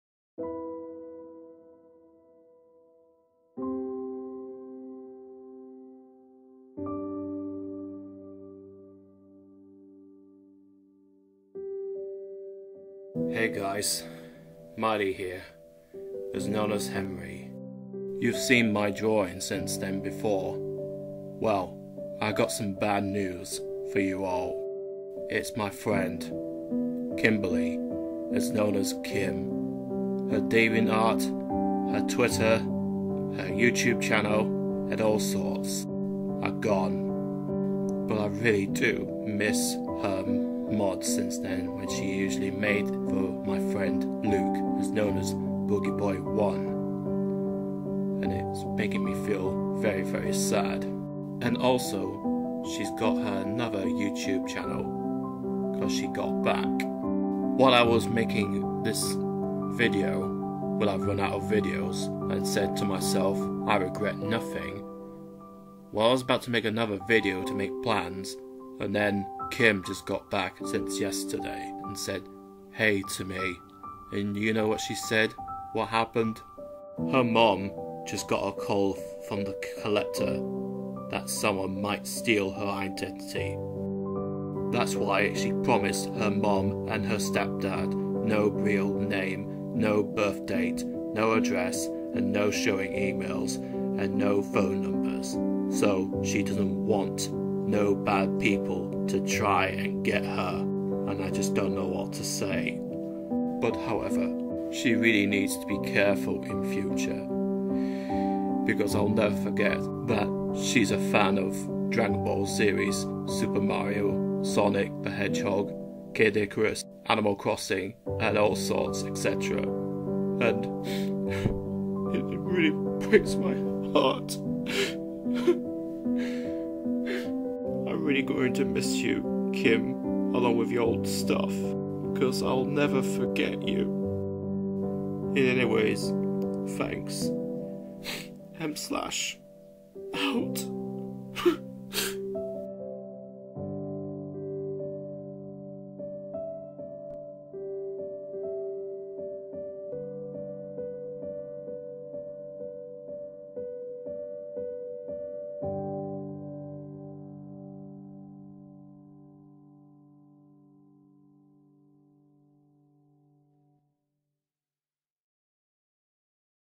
I'm going to miss you Kimberly
Here's my Art that I just made since before.

I know what's happen since yesterday but she has lost all of her sites since before. And it's sad while everyone's upset. And for now she's got back on Youtube. Poor Kim.

You see, she did all of these characters, weapons and stages from MMD since then and after that she stopped.
Also she has lost his OC Dragon Ball Character Gloom was well from last few years ago.

Mighty Craft Slash belongs to Me
Kimberly (Gi) belongs to AnimeGirl